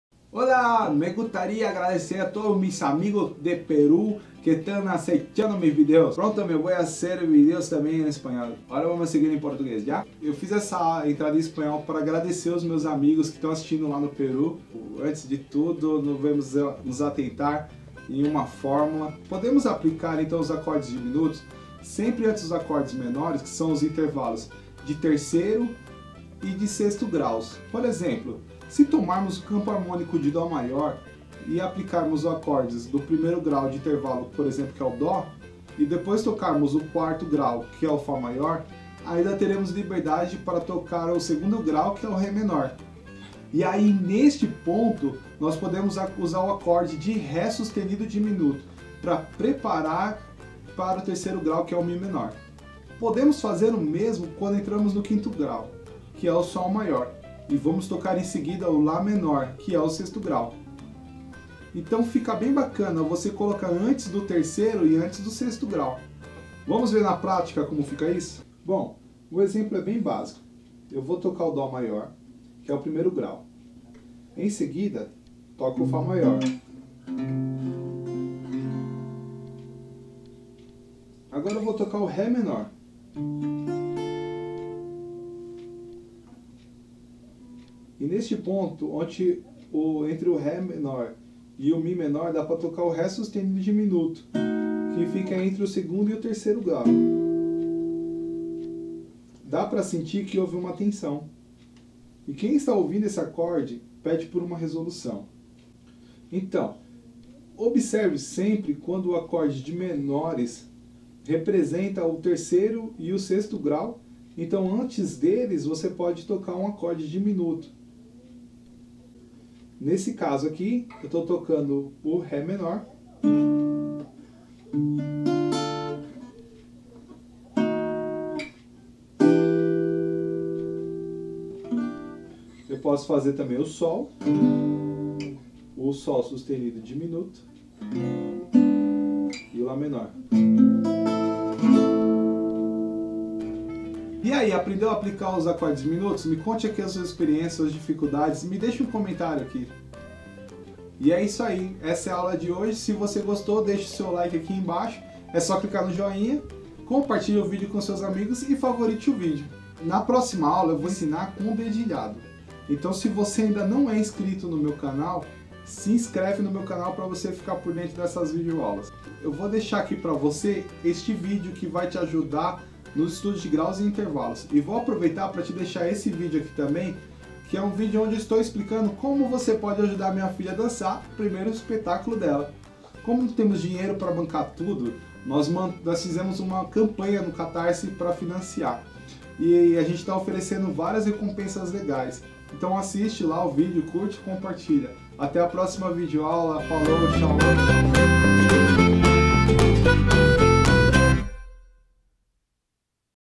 Como Aplicar Acordes Diminutos | Aulas de Violão (VIEP016)
Olá, neste vídeo eu me empenho a ensinar em poucos minutos você aplicar os acordes diminutos em uma canção ou composição.
Se você ainda não assistiu a vídeo aula de como fazer acordes diminutos assista neste
Assista ao vídeo para aprender a empregar acordes diminutos e se torne um violonista mais top.